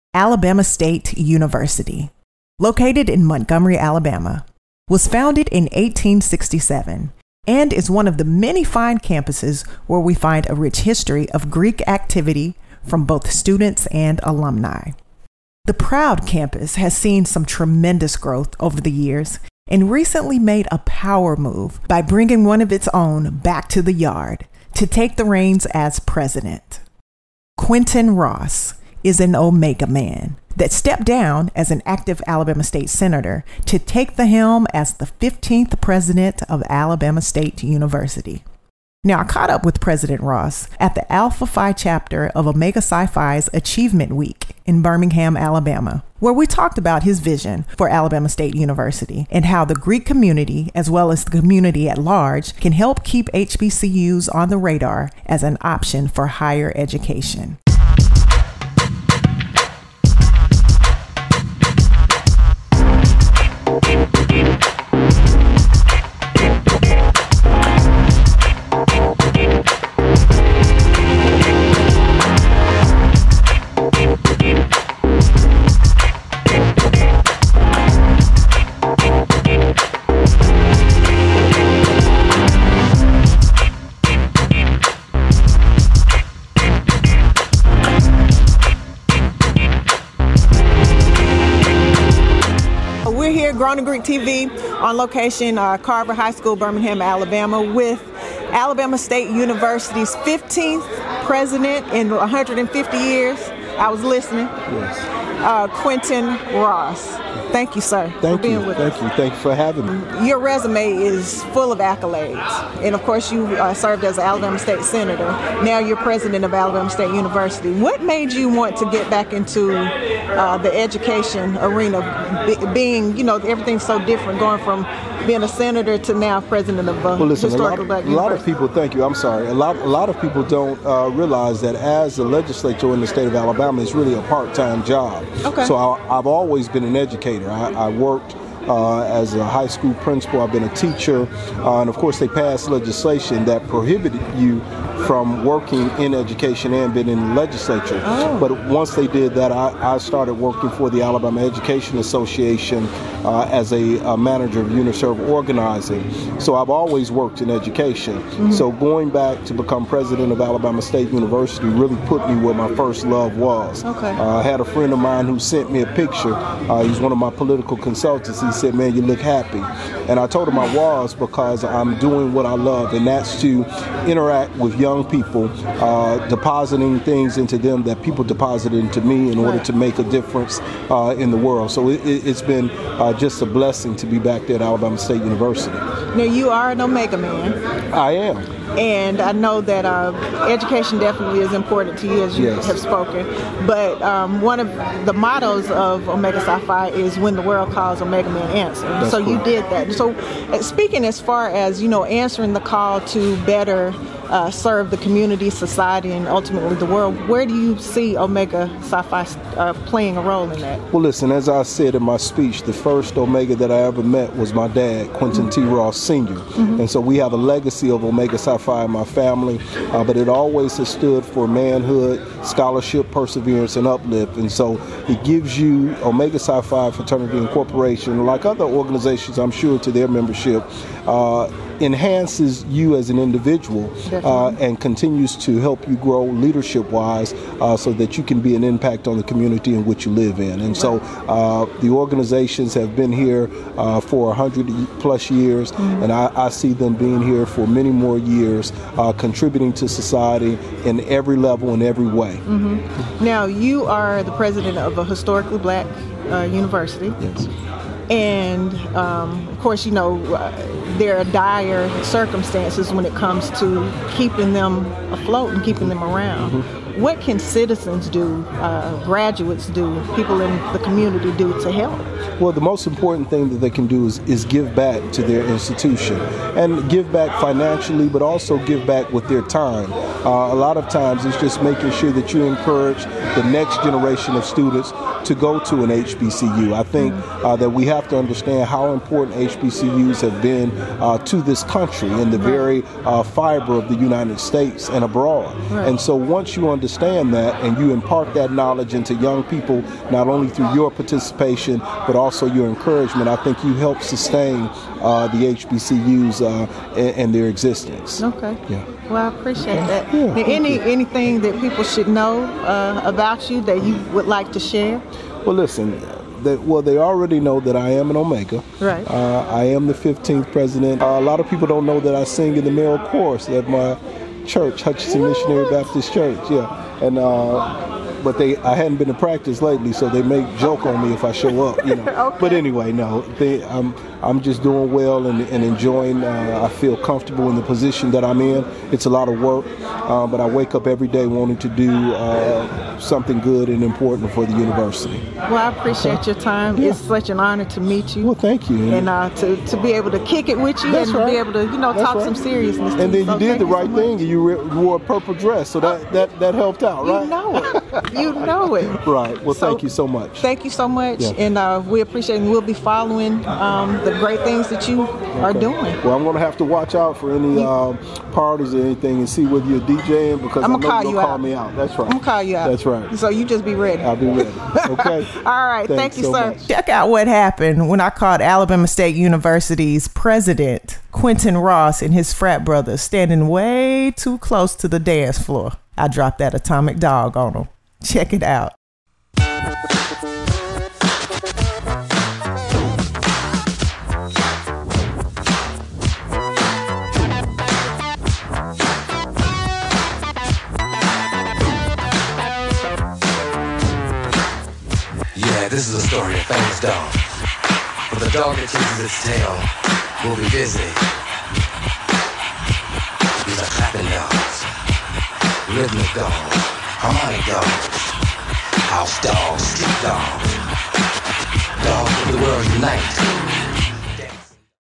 Dr. Quinton T. Ross, Jr. - Omega Psi Phi - (Grown and Greek TV)
Phaith catches up with the President of Alabama State University and Omega Man, Dr. Quinton T. Ross, Jr. as he served as the guest speaker for the Alpha Phi Chapter of Omega Psi Phi Achievement Week Dinner in Birmingham, Alabama.